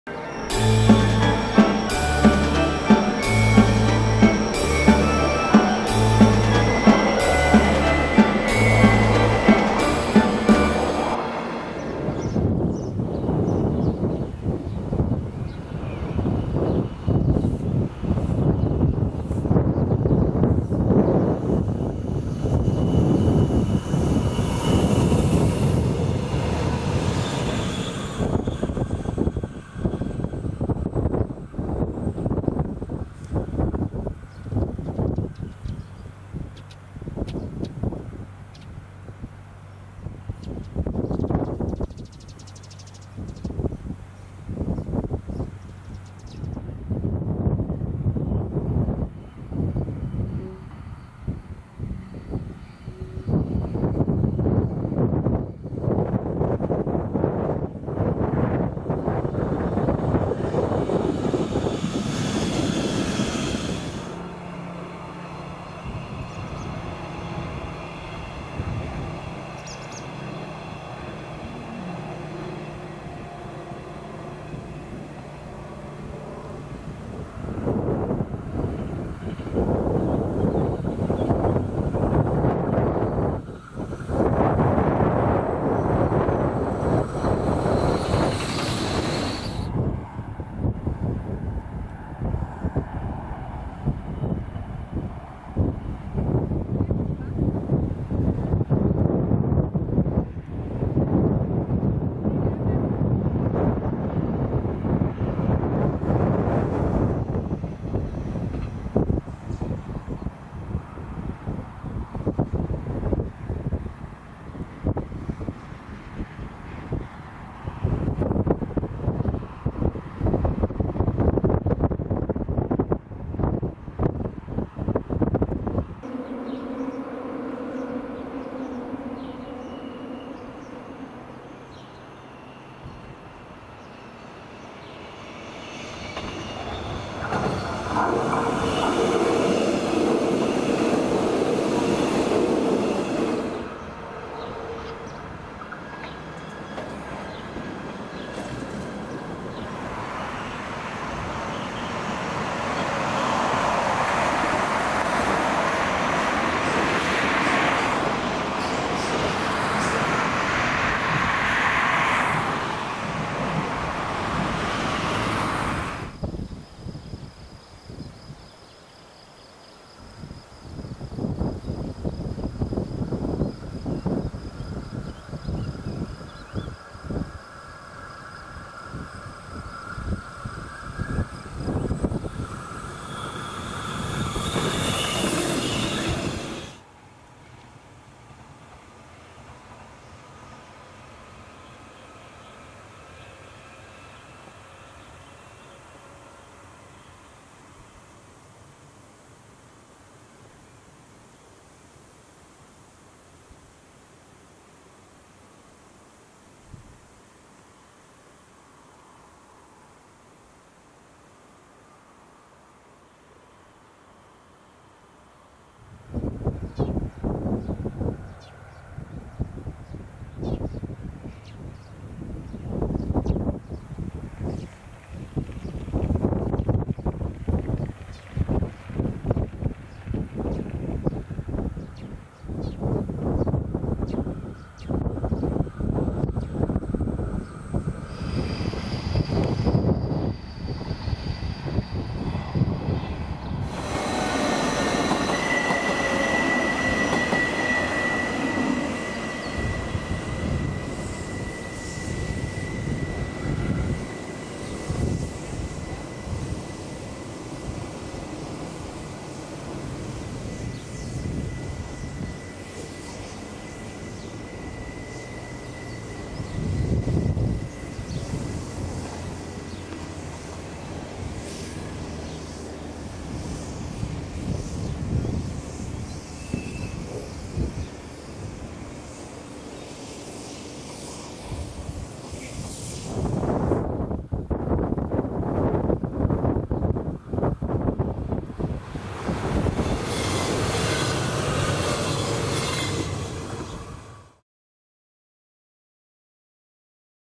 RBS | Regionalverkehr Bern-Solothurn | Swiss narrow gauge trains | Automotices RABe 4/12 21-34 NeXT
[FR] RBS, Regionalverkehr Berne - Soleure, Automotrices série RABe 4/12 N° 21 à 34. "NEXT" Niederflur-Express-Triebzug (Automotices express à plancher surbaissé.
[DE] RBS, Regionalverkehr Bern - Solothurn, Triebwagen der Baureihe RABe 4/12 Nr. 21 bis 34. "NÄCHSTER" Niederflur-Express-Triebzug.
[EN] RBS, Regionalverkehr Berne - Solothurn, RABe 4/12 Series Railcars No. 21 to 34. "NEXT" Niederflur-Express-Triebzug (Low-floor express railcars.